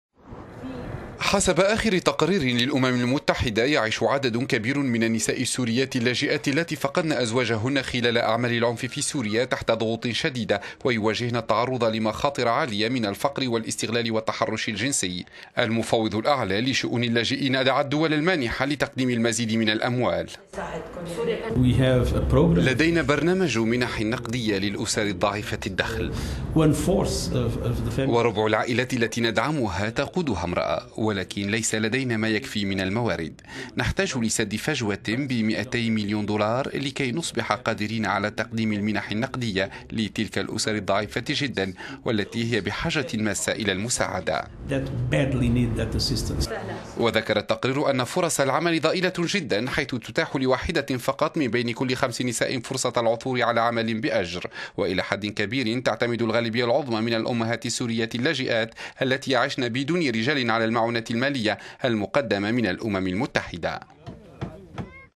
اللاجئات السوريات يواجهن الإذلال والفقر والتحرش
حسب تقرير للأمم المتحدة يعيش عدد كبير من النساء السوريات اللاجئات اللاتي فقدن أزواجهن خلال أعمال العنف في سوريا تحت ضغوط شديدة، ويواجهن التعرض لمخاطر الفقر والإستغلال والتحرش الجنسي. وأوضح التقرير الصادر عن مكتب المفوض الأعلى للأمم المتحدة لشؤون اللاجئين في جنيف أنه من بين كل أربع أسر للاجئين السوريين في مصر ولبنان والعراق والأردن توجد أسرة تقودها إمرأة، مما يعني أن نحو مائة وخمسة وأربعين ألف امرأة في هذه الدول يواجهن هذا الوضع. ودعا المفوض الأعلى لشؤون اللاجئين الدول المانحة لتقديم المزيد من ال...